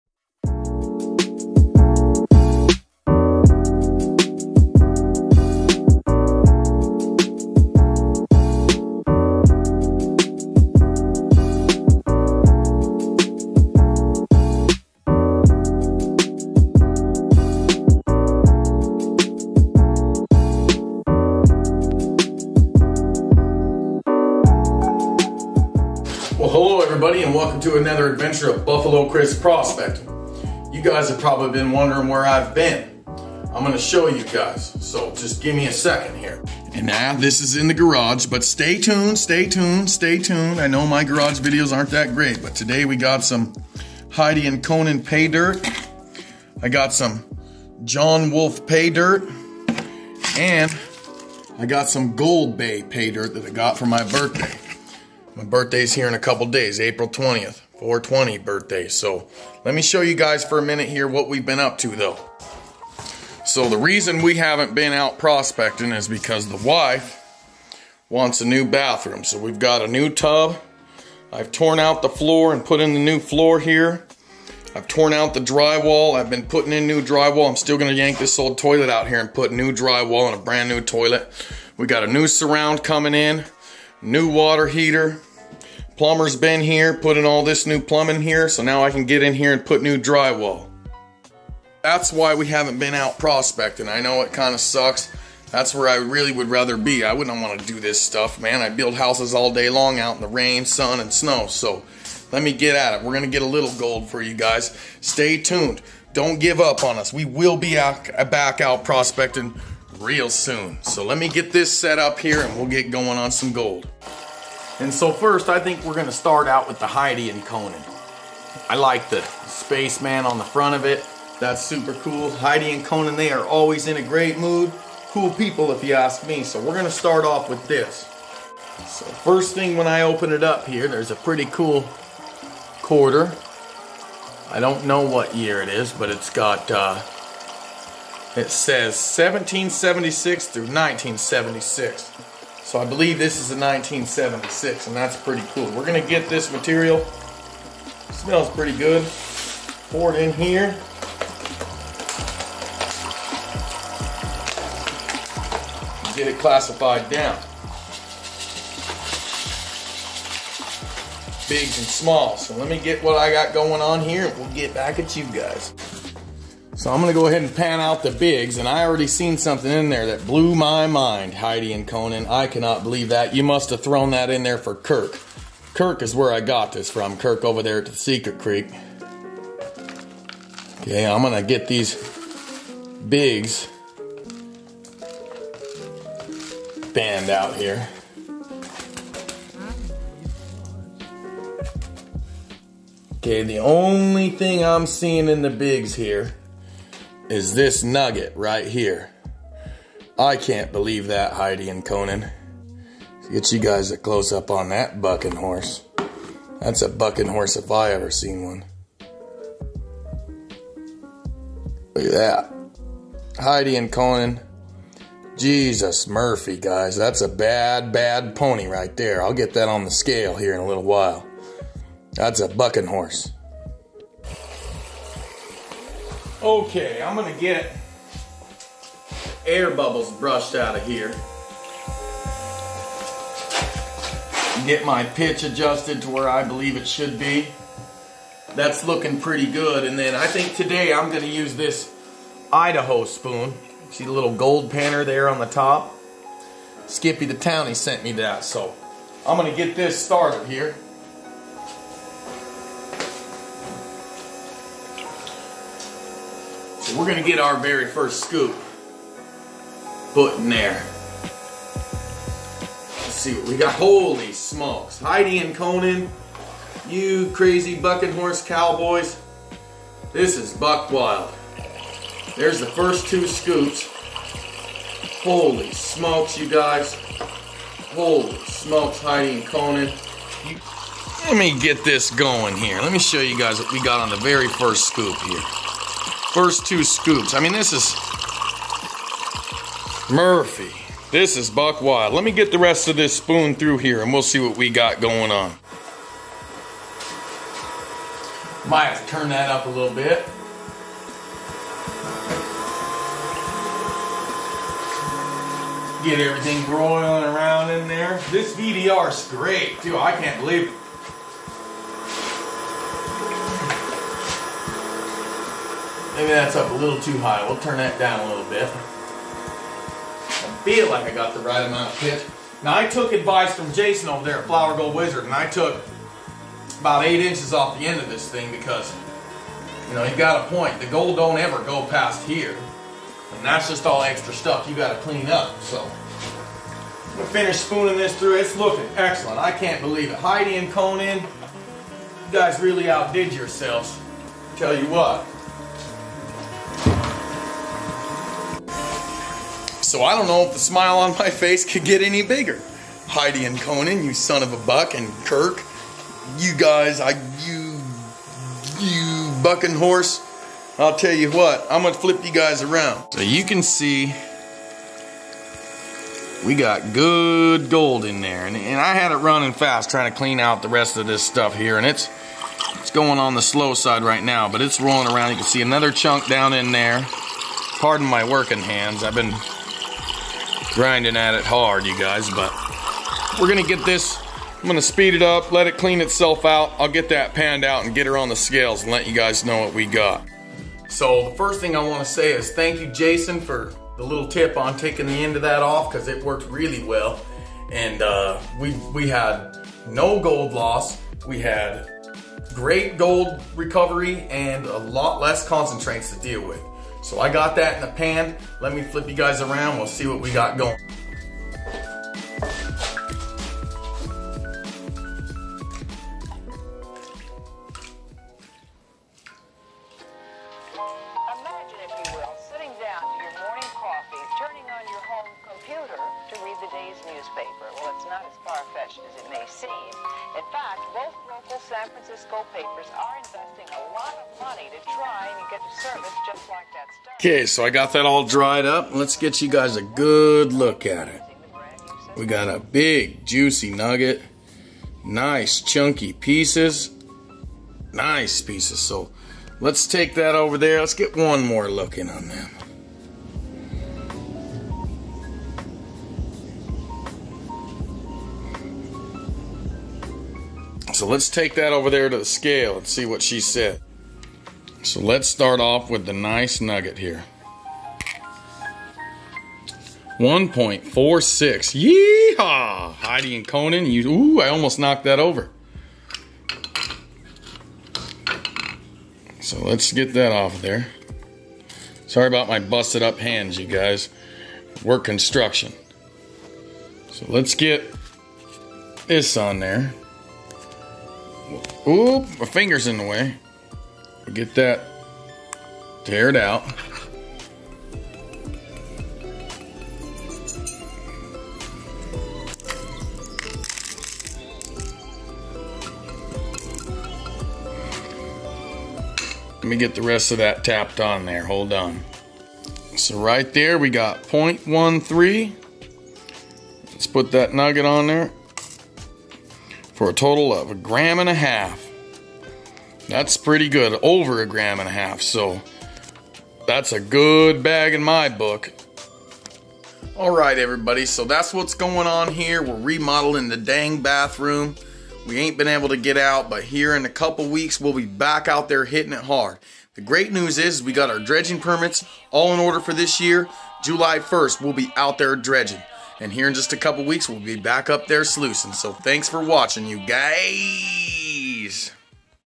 NO GOLD PROSPECTING?????? Heidi and conan gold nugget. and what's going on?
I haven't been able to go gold prospecting lately because I'm remodeling my bathroom for my lovely wife. I promise in a couple weeks I'll be back at it. hang in there